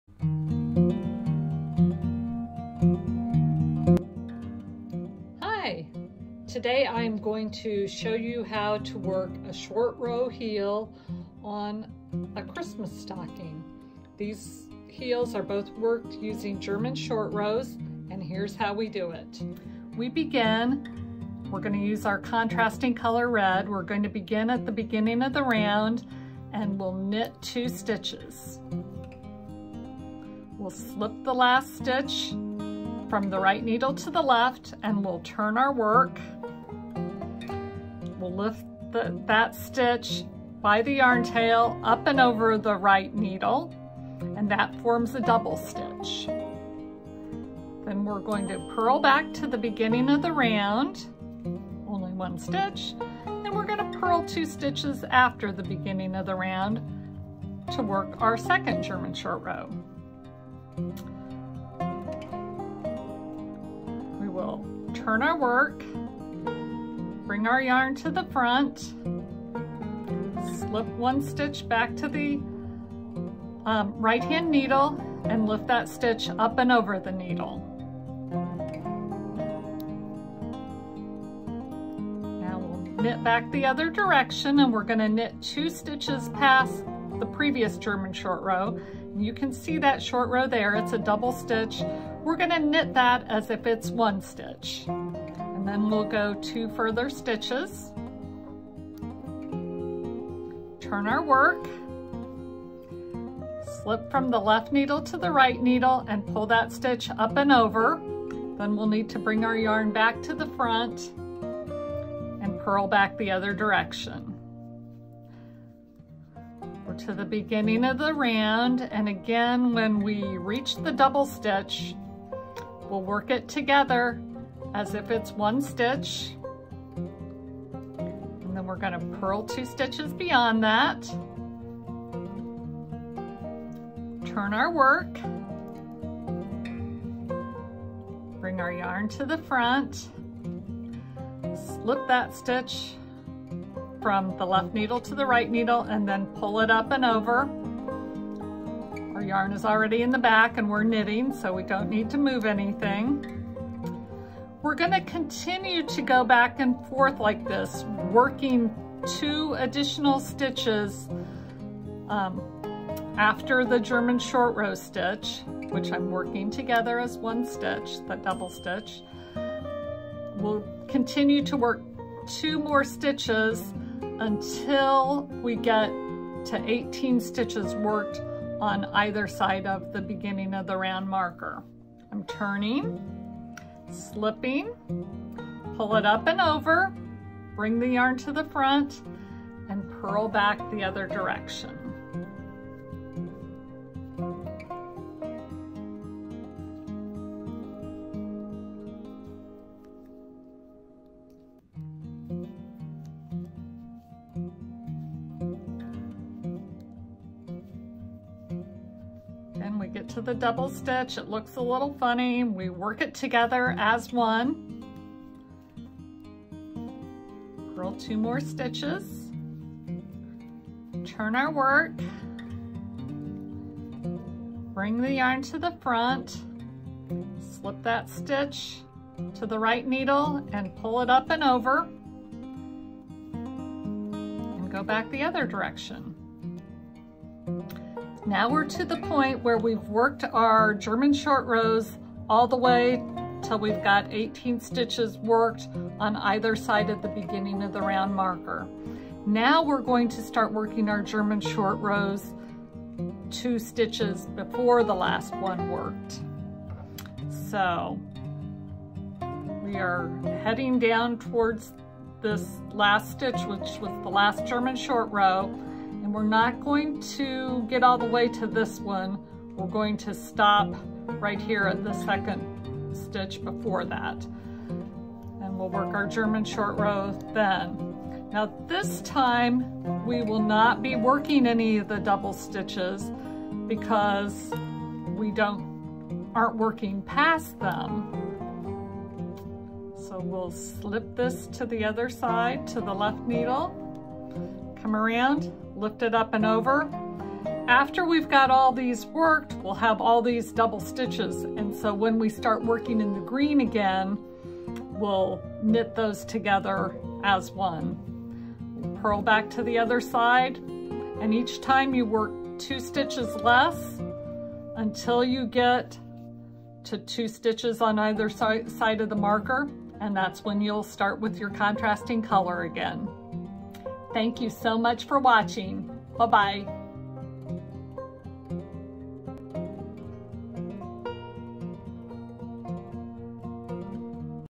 German Short Row Heel on a Christmas Stocking
This knitting tutorial from Wise Owl Knits, formerly Cheryl Beckerich Knits, demonstrates how to add a German Short Row Heel on a Christmas Stocking.